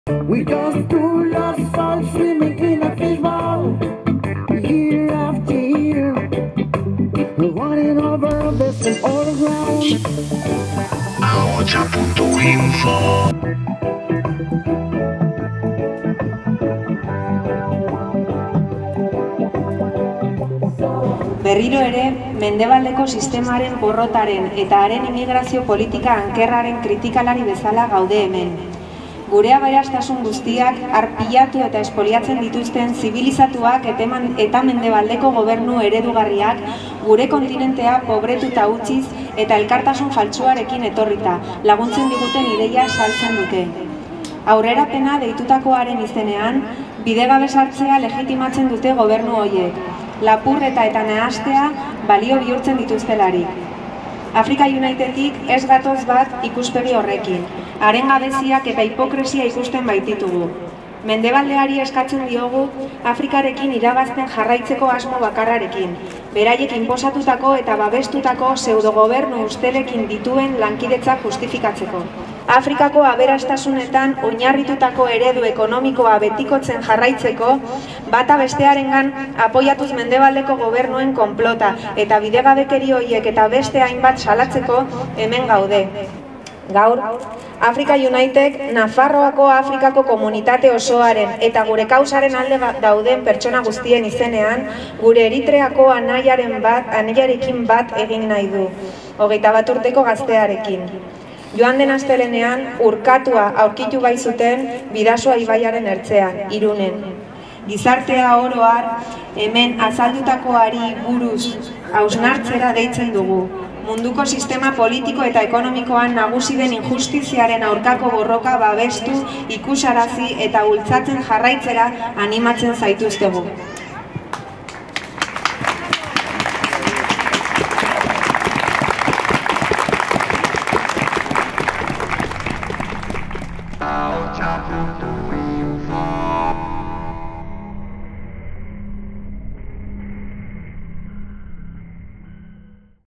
Mendebaldeak Afrikako egoera lazgarrian duen erantzukizuna salatu du Africa United-ek
Aste honetan Irun eta Hendaia arteko mugan urkatu den Eritreako gazte etorkinaren gogoratzeko ekitaldia egin zuen atzo Africa United elkarteak Iruñean. Mendebaldeko herrialdeek, eta bereziki Frantziak, Afrikako egoeran duten erantzukizuna salatu zuten.